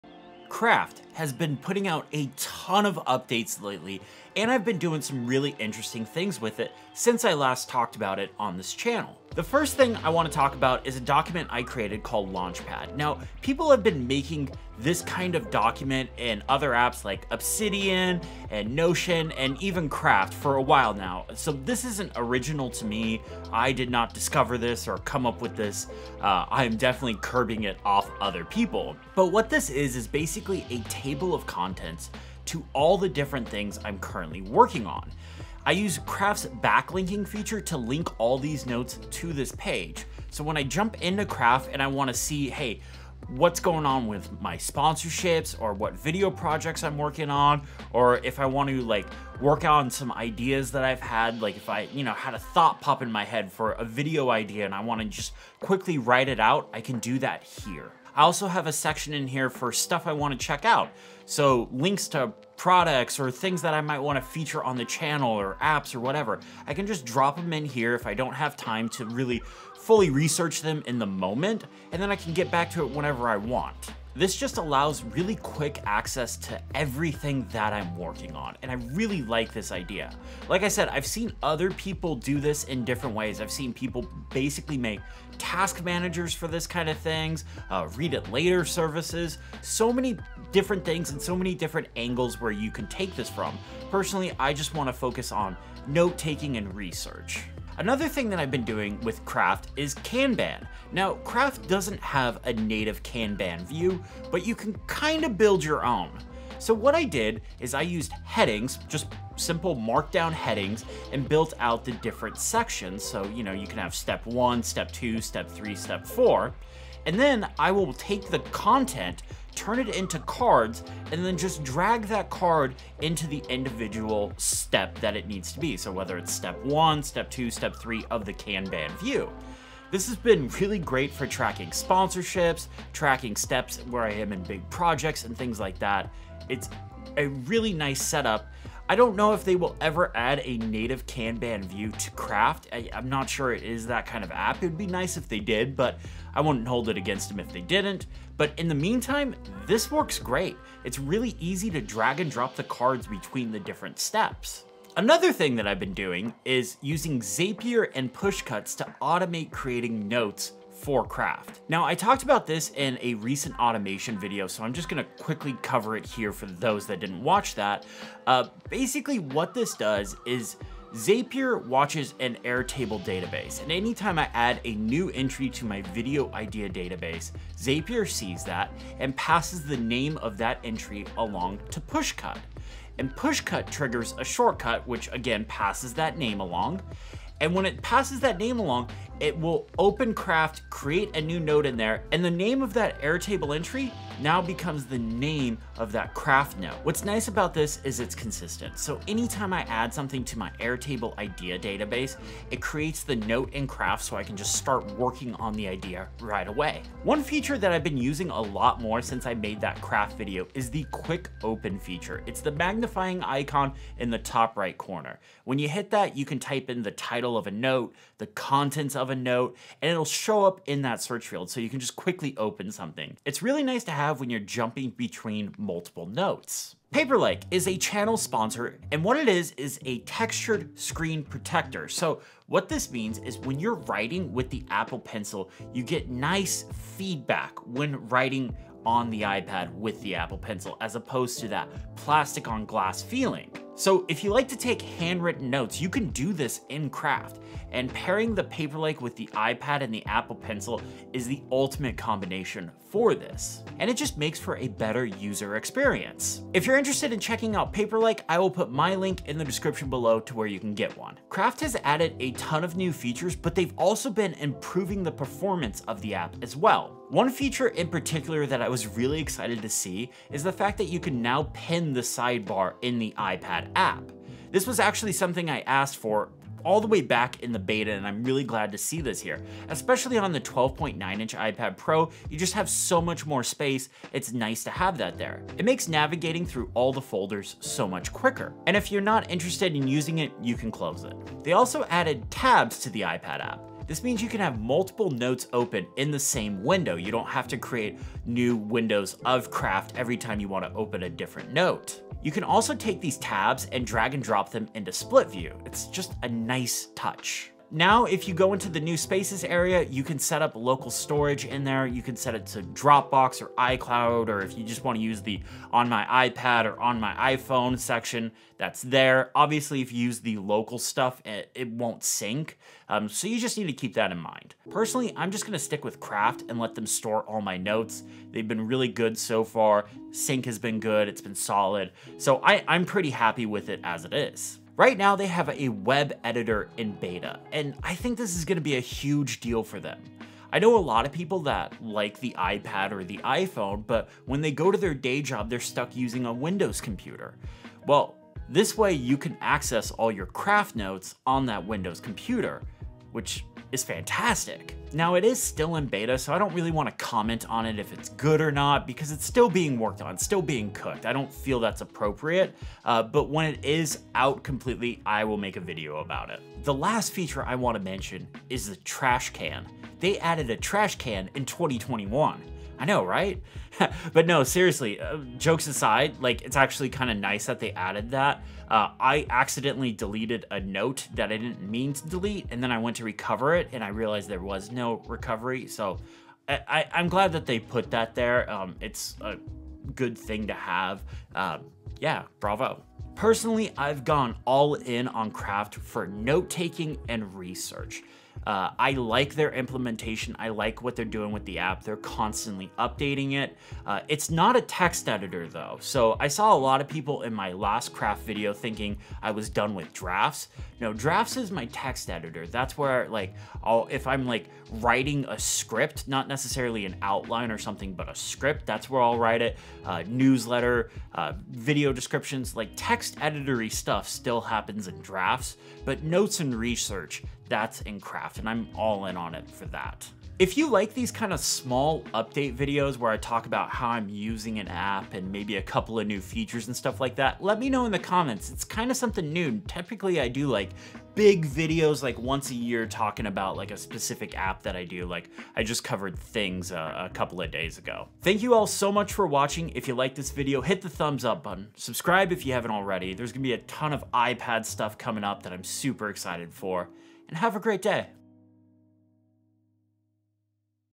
Tips for Using Craft & New Features
Let's talk about Craft updates and how I've been using it.

## Links

## More Links